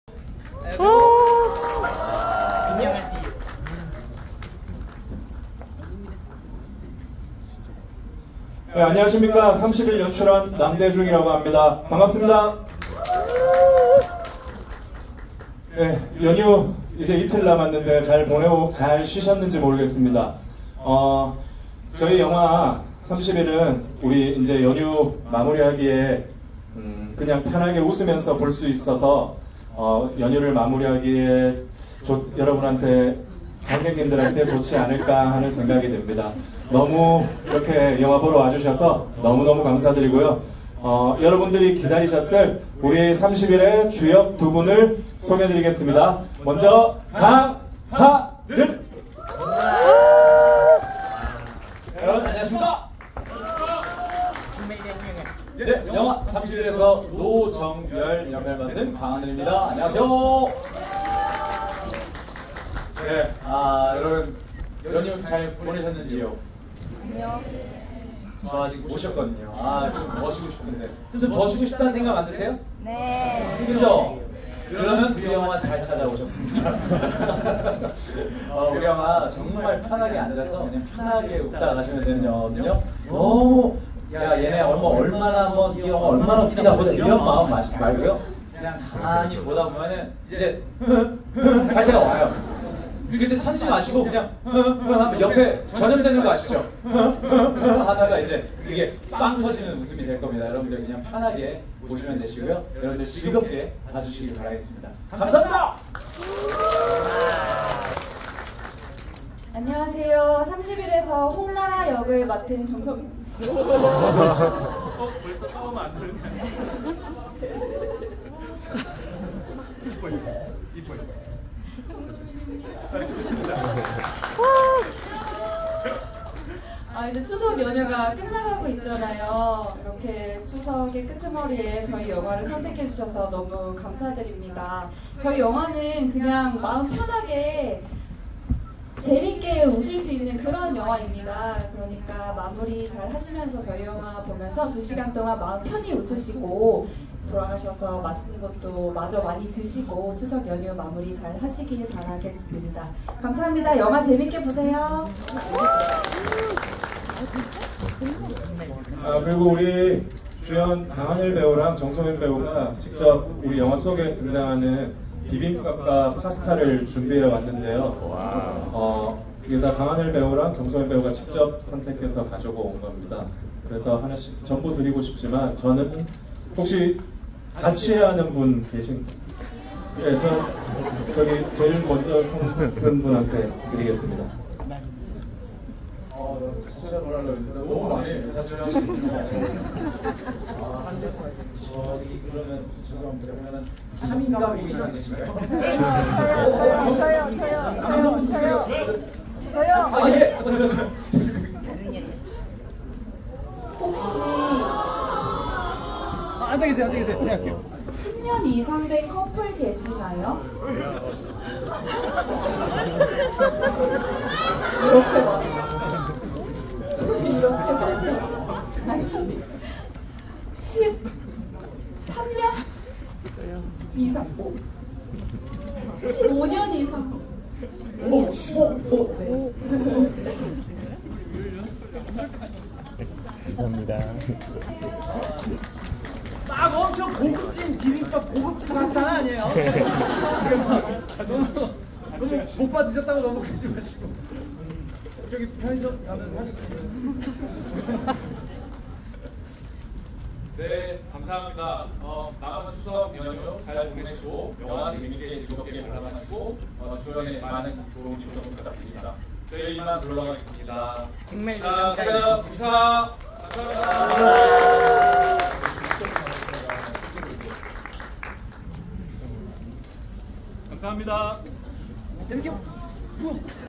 💓30일💓Love Reset 개봉전 무대인사 강하늘 정소민 남대중감독 (메가박스 이수)
♡Billup Hop♡
♡빌럽홉♡